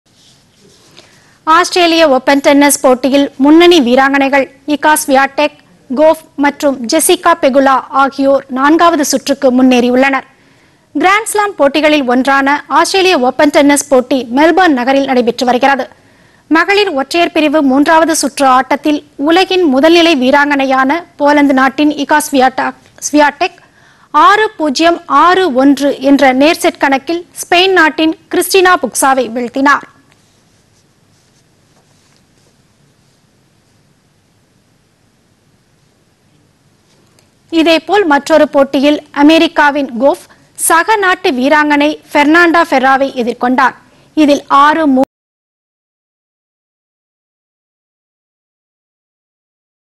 ஆஸி. ஓபன் டென்னிஸ் போட்டிPodhigaiTamilNews
ஆஸ்திரேலிய ஓபன் டென்னிஸ் போட்டி - முன்னணி வீராங்கனைகள்  Swiatek, Gauff, Pegula அடுத்த சுற்றுக்கு முன்னேற்றம்